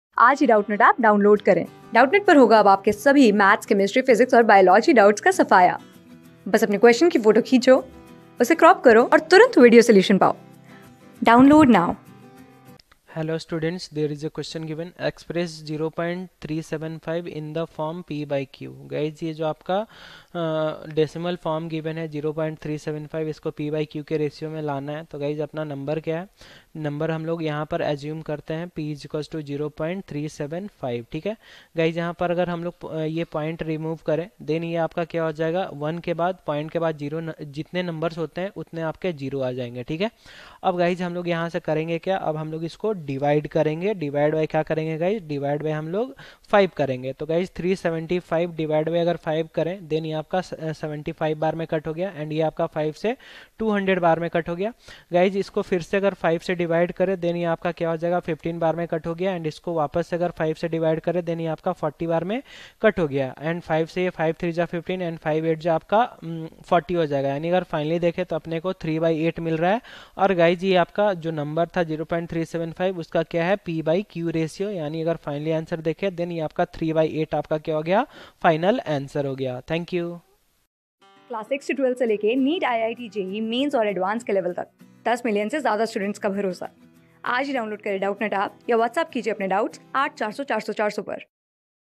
Express 0.375 in the form (p)/(q). | CLASS 10 | BOARD QUESTION PAPER (SOLVED) - 2019 | MATHS | D...
Express 0.375 in the form (p)/(q).
Class: 10
Subject: MATHS
Chapter: BOARD QUESTION PAPER (SOLVED) - 2019
Board:Haryana Board

👉 Have Any Query? Ask Us.